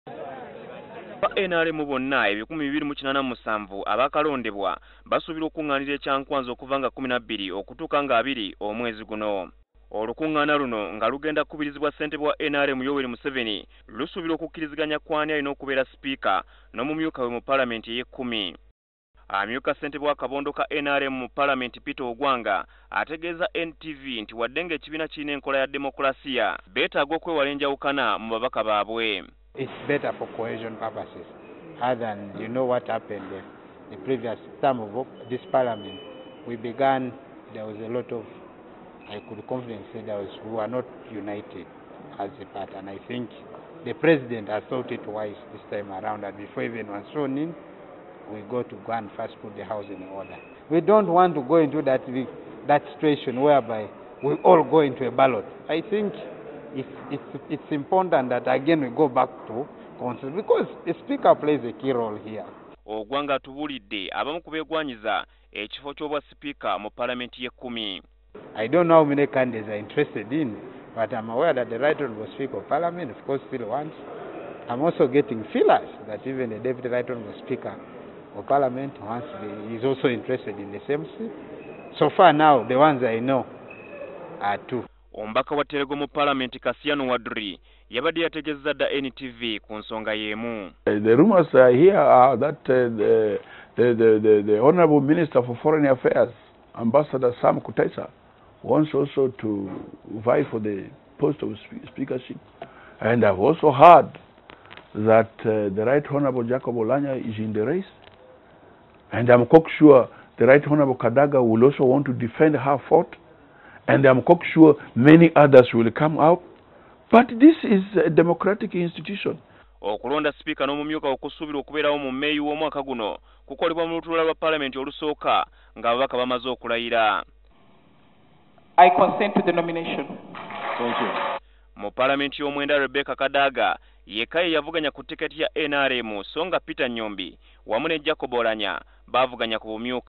Ekifo ky’obwa sipiika: Ababaka ba NRM baakusisinkana e Kyankwanzi
Ssentebe wa NRM Yoweri Museveni ayise olukiiko lwa’ababaka ba palamenti aba NRM olugenda okumala ennaku 9 e Kyankwazi.

Tutegedde okuva ew’omuyuka w’akulira akabondo ka NRM Peter Ogwang nti ekimu ku bigenda okuteesebwako y’ensonga y’ani anabeera siipika n’omumyuka we.  Ebirala ebigenda okuteesebwako  kuliko manifesito ku butya bweyinza okuteekebwa mu nkola. Rebecca Kadaga n’amumyuka Jacob Oulanyah basuubirwa okuddamu okuvuganya ku bifo bino.